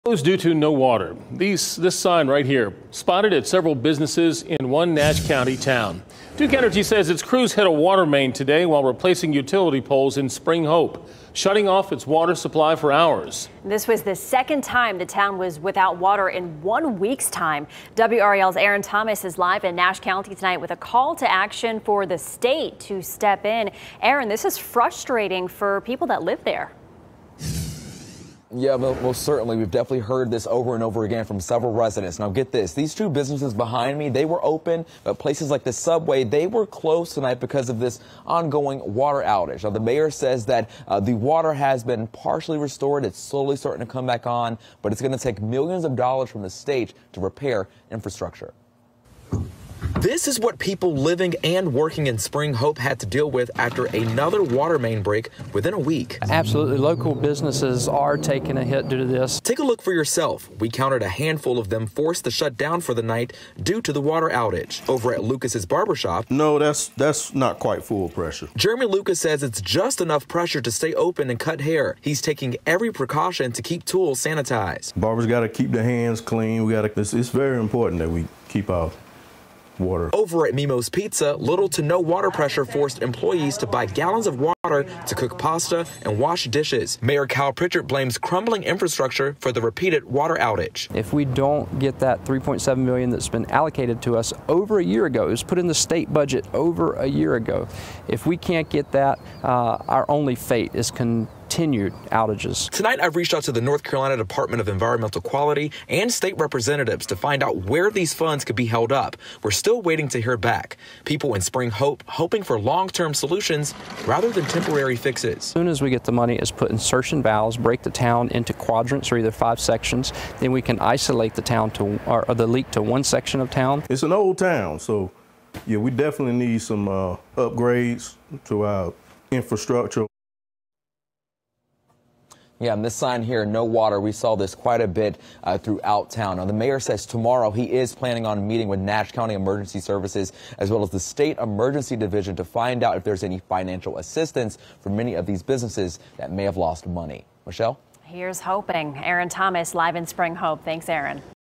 Spring Hope businesses feel the pinch of closures due to water outages
Spring Hope was without water for the second time in one week after Duke Energy's crews hit a water main on Thursday while replacing utility poles.